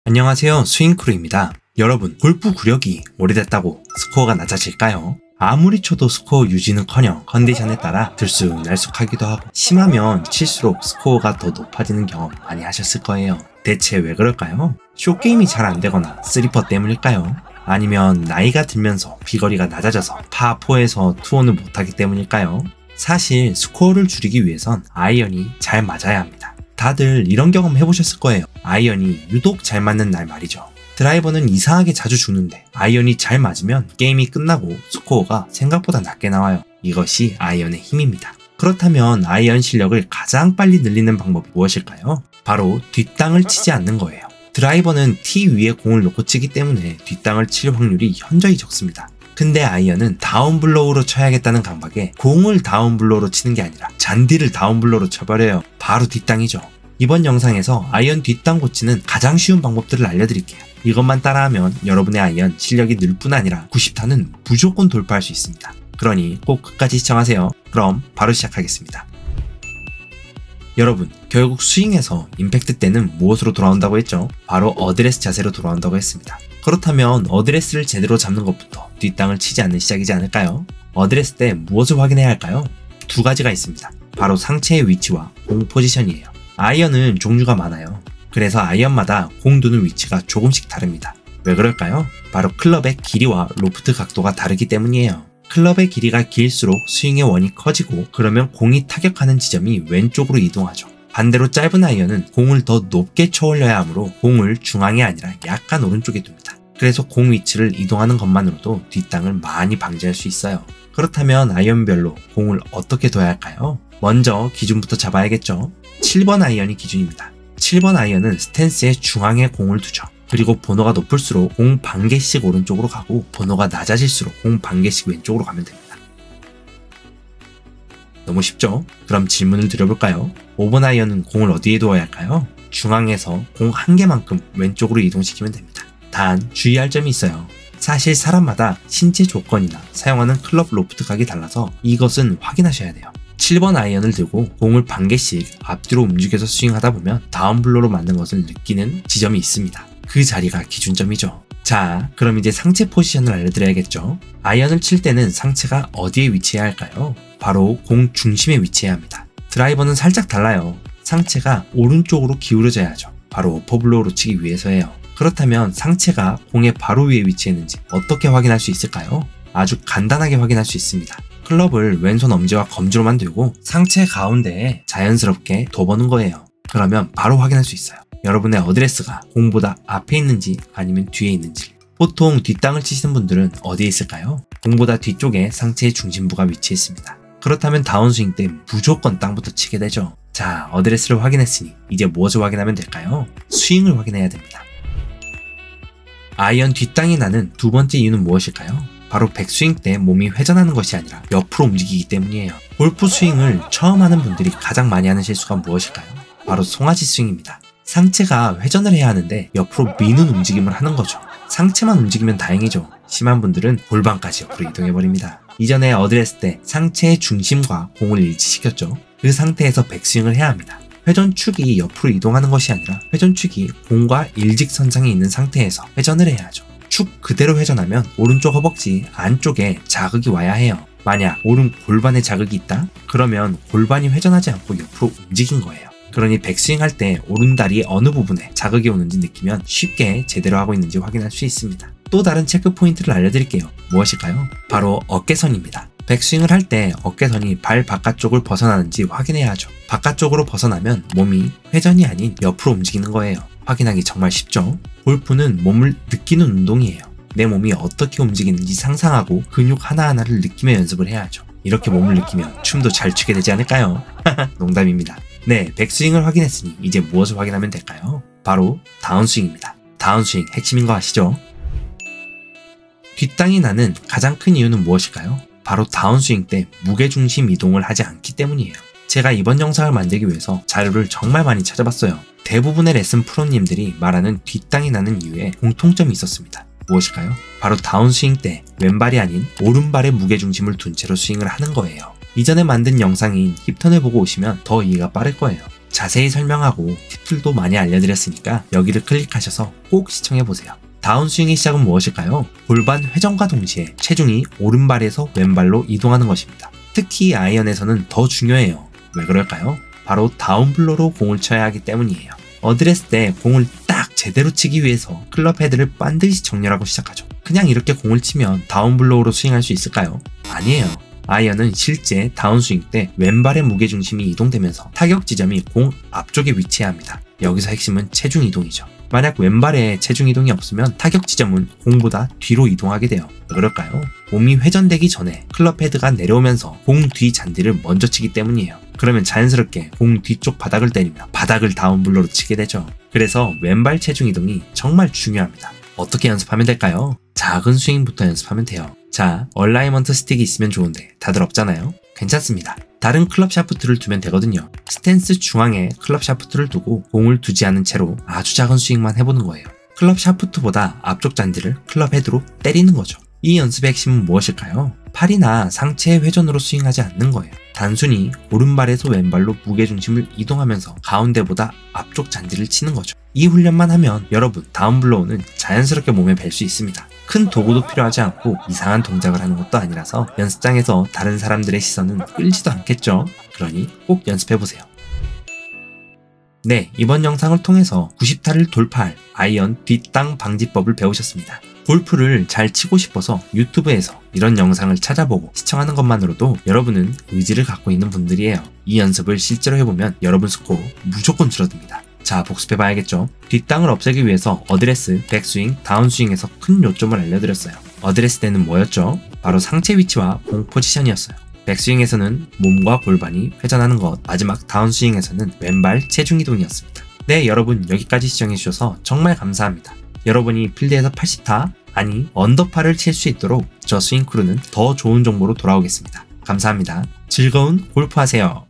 우드, 드라이버 보다 아이언을 잘 치기 위해선 이것을 해야 합니다.
▼▼7번아이언 툭툭쳐도 150m보내는 법▼▼

안녕하세요! 스윙크루입니다!

여러분 싱글을 치기위한 가장 쉬운방법이 무엇일까요?

바로 아이언입니다.

아이언이 잘 맞는날은 드라이버가 안맞더라도 충분히 타수가 줄어들죠.

이번영상을 끝까지 시청하시고 아이언 실력이 일취월장하는 구독자님들이 되길 바랍니다.

구독자님들이 싱글을 치는 그 날까지 저 스윙크루는 더 열심히 준비하겠습니다!

감사합니다. 여기까지 스윙크루였습니다!

0:00 아이언 뒷땅만 안쳐도 싱글 가능합니다!
1:09 뒷땅 방지는 어드레스부터 시작합니다!
3:16 백스윙 때 상체와 골반이 “회전”해야 합니다!
4:32 다운스윙 때 체중이동을 해야 합니다!
6:25 아이언 다운블로우는 누구나 가능합니다!

24. midform